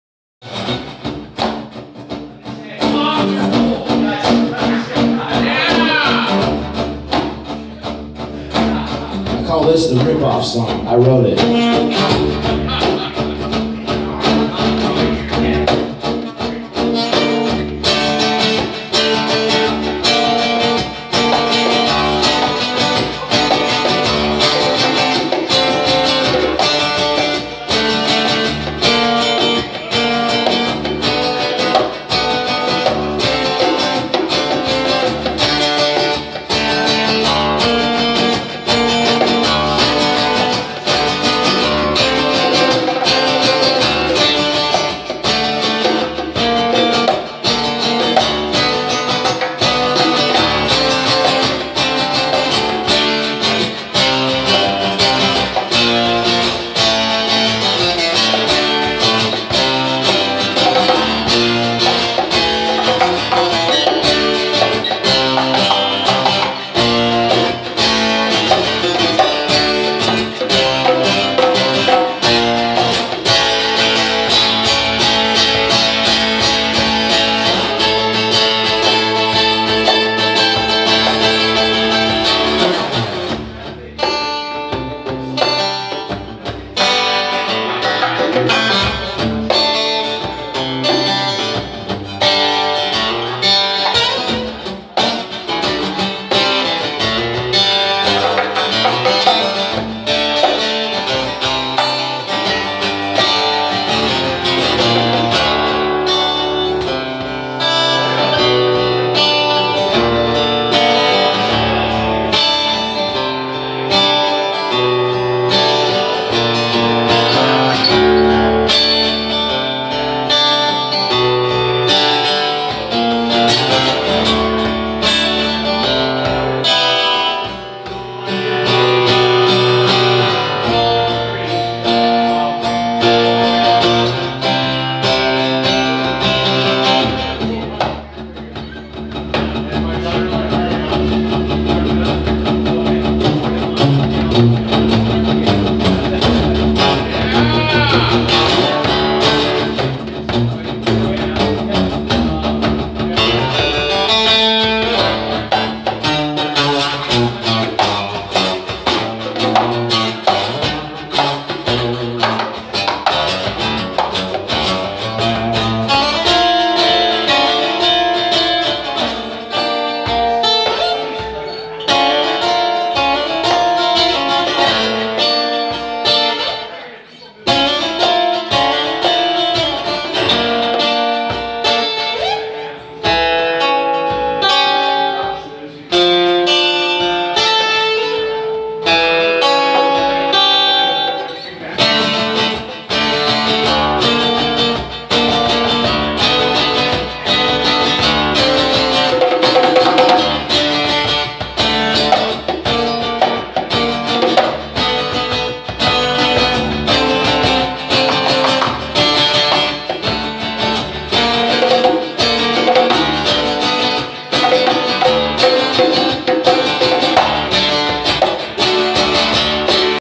Ryan Marchand with Germain • Open Mic at The Vets
It's always nice when we can coax the guy running sound at the OPEN MIC to also play something for us. just whatever he feels in the moment. with JIMMY DUCK's GUITAR. "I call this the rip-off song. I wrote it" -Ryan Marchand